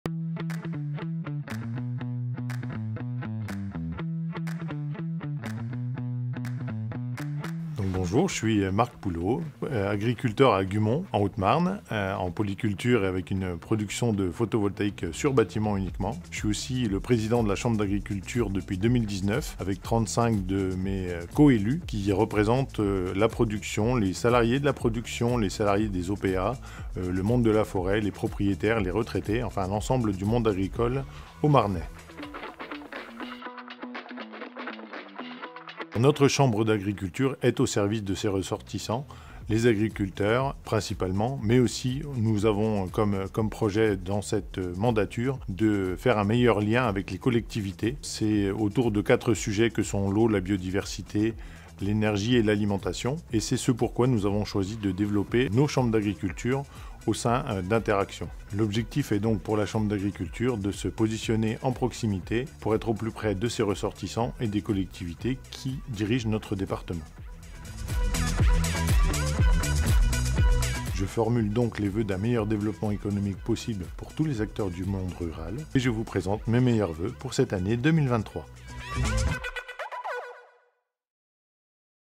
voeux2023 | Marc POULOT, Président de la Chambre d'agriculture de Haute-Marne✨
Marc POULOT, Président de la Chambre d'agriculture de Haute-Marne, vous présente ses vœux pour cette nouvelle année.